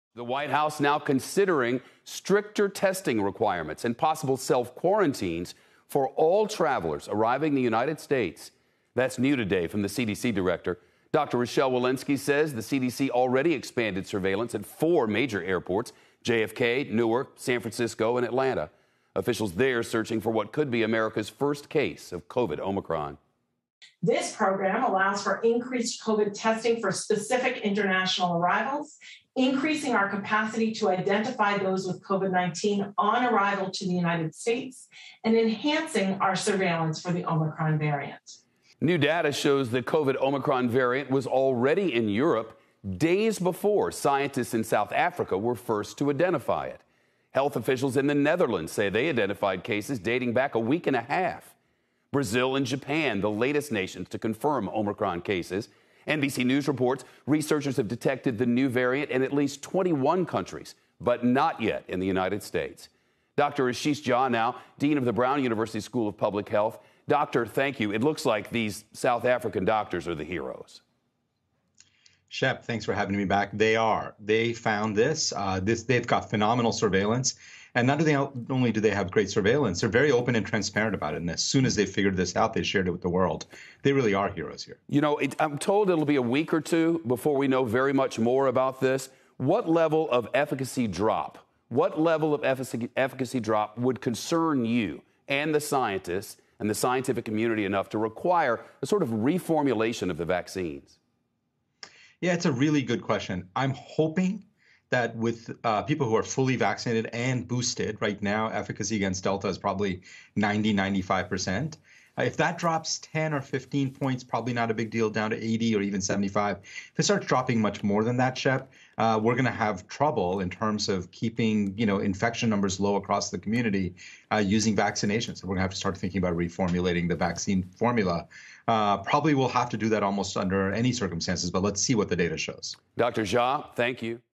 White House considers stricter testing requirements for travelers
Brown University School of Public Health Dean Dr. Ashish Jha joins Shep Smith to discuss the omicron Covid variant and just how impactful current vaccines could be against it.